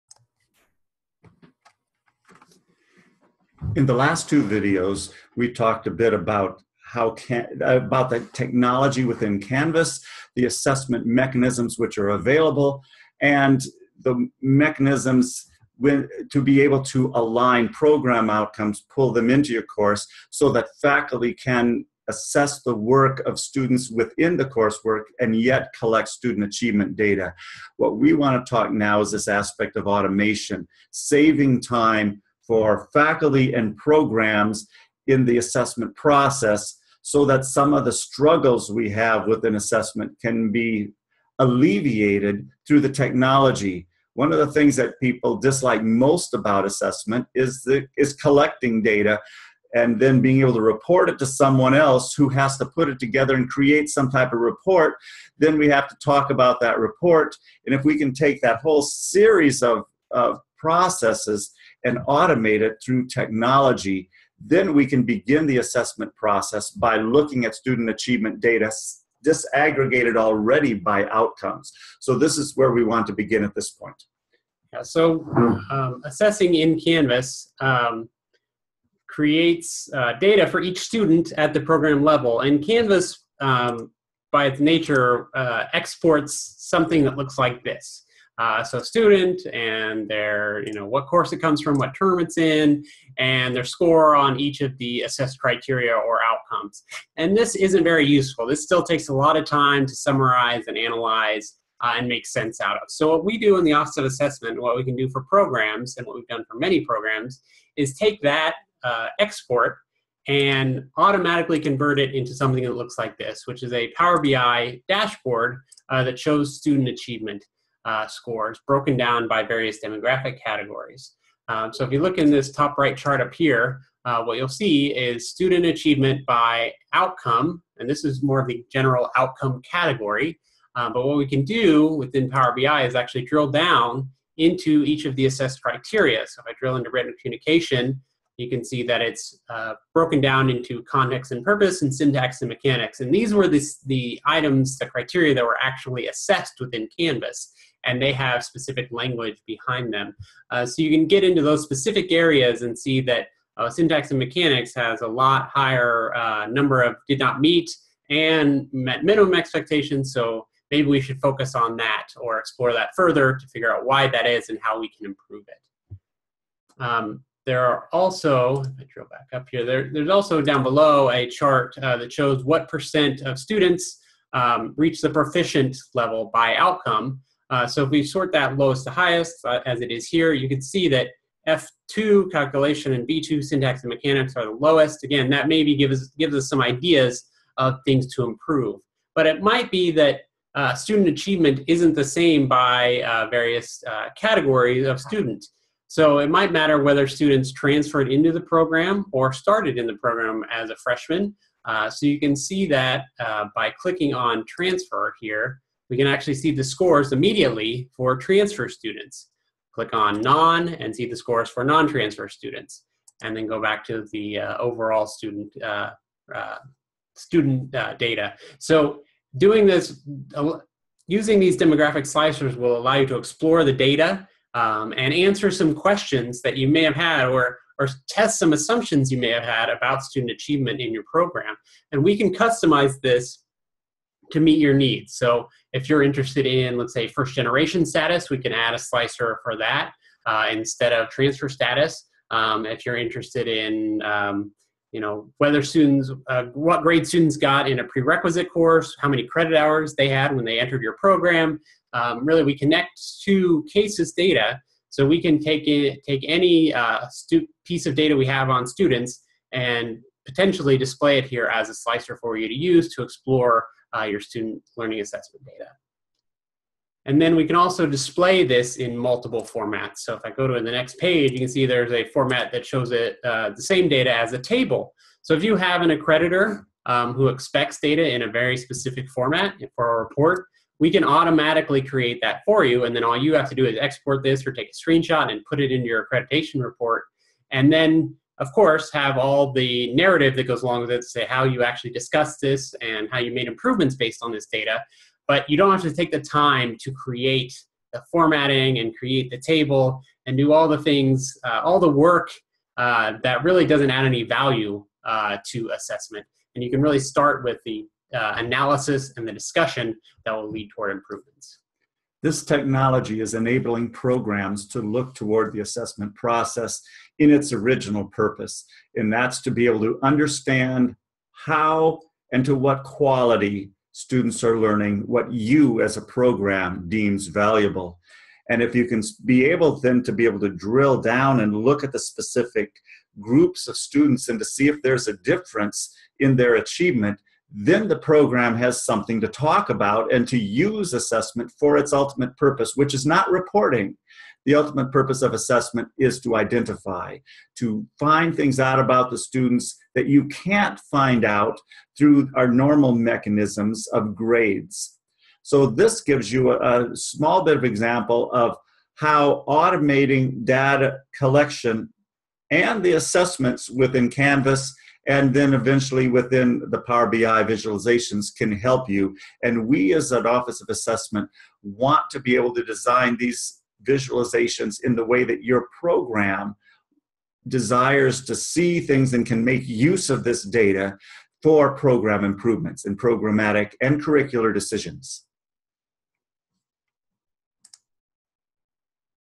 Canvas Power BI Reporting
Learn about Power BI and it's powerful and appealing interface used for it's ability to distinguish trends.